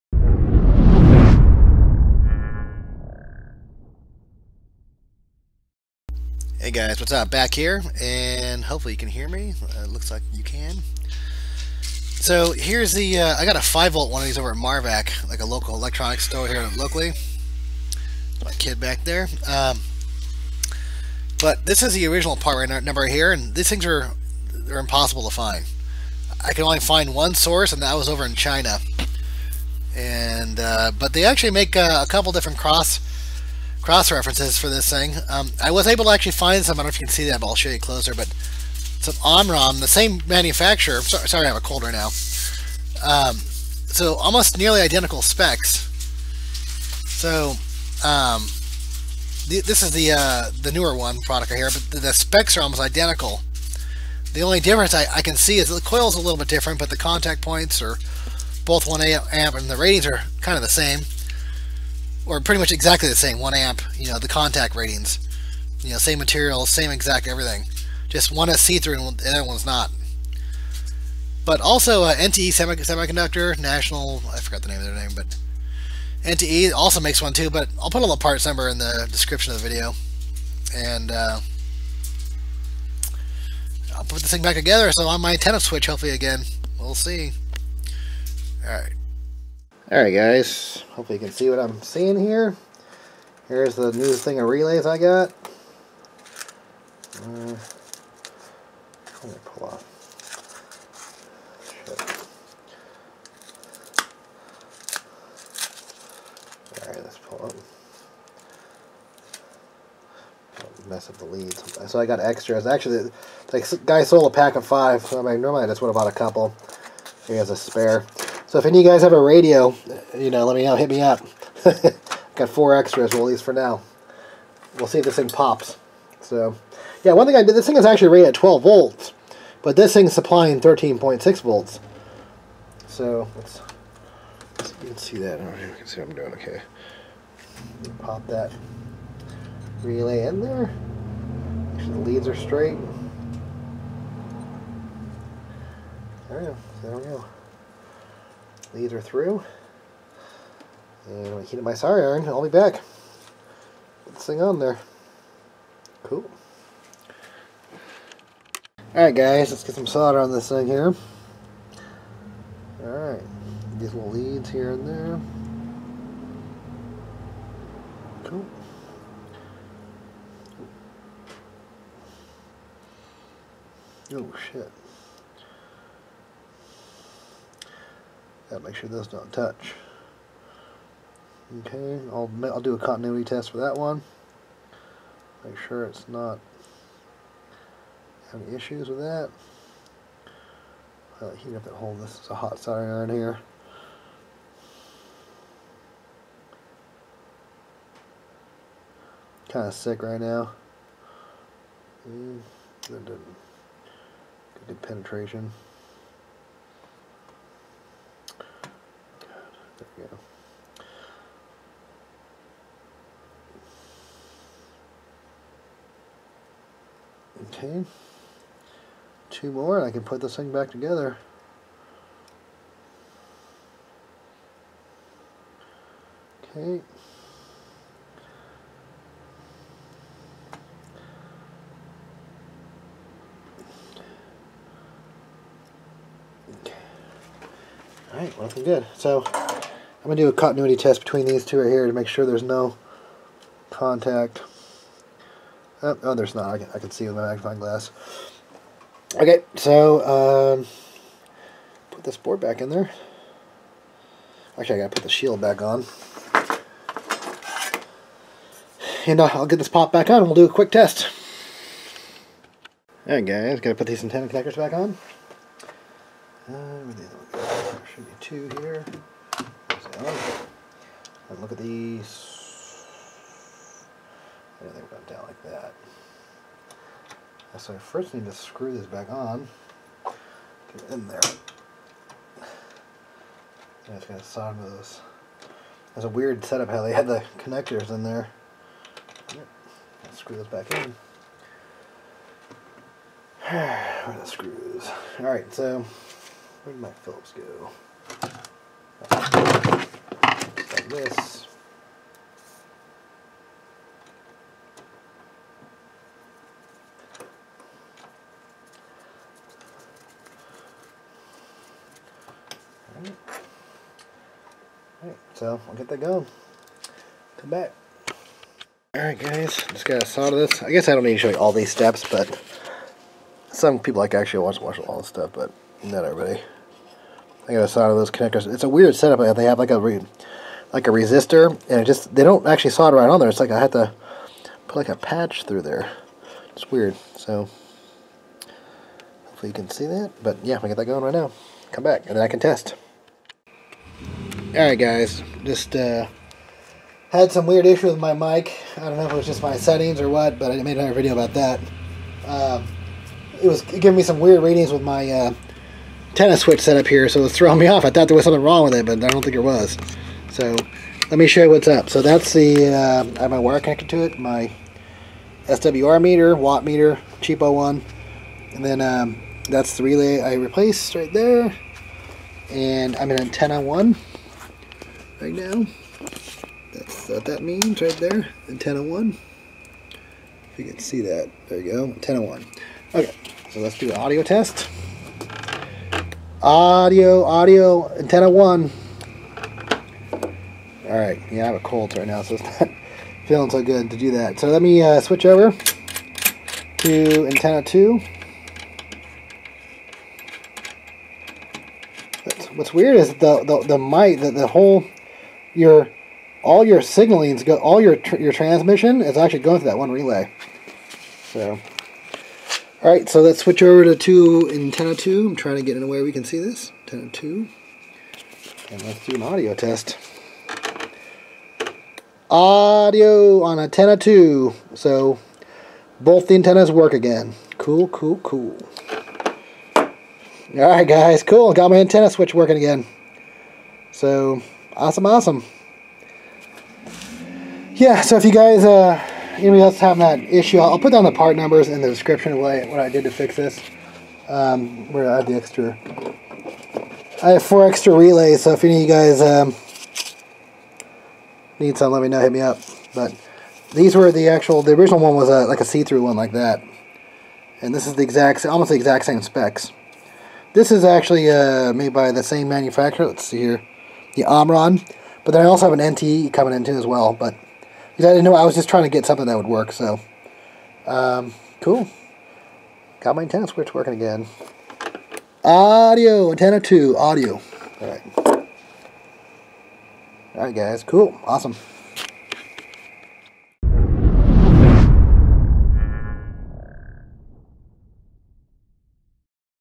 Icom IC-736 internal Antenna switch repair bad relay fix part 2 yaesu kenwood
My internal antenna switch was not working on my icom transciever. Its was stuck on antenna 1 and would not switch to antenna 2. I decided to do a little troubleshooting and traced it back to a bad relay. The original replacement relays are discontinued and very to find.

Original Relay part number = Omron G5A-237P-DC12
              replacement part = Omron G5A-234PHDC12-ND
             NTE cross reference = R72-11D1-12C If you found this video helpful or would like help fund future experiments. please donate below.